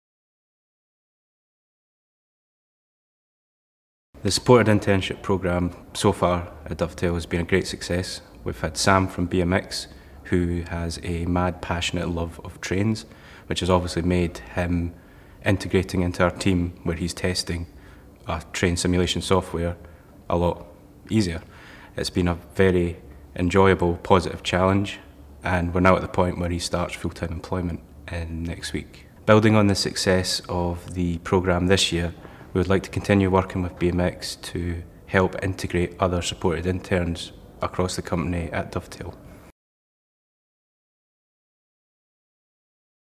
Supported Internships with Dovetail Games
Are you a business in Medway?

If you are, we are looking for businesses to join our Supported Internship programme to help young adults with learning difficulties and/or autism find a role they enjoy.

Only 6% of adults with a learning disability known to their local authority in England are in paid work (NHS Digital 2018) and we want to change that.

Get in touch to find out more or start the ball rolling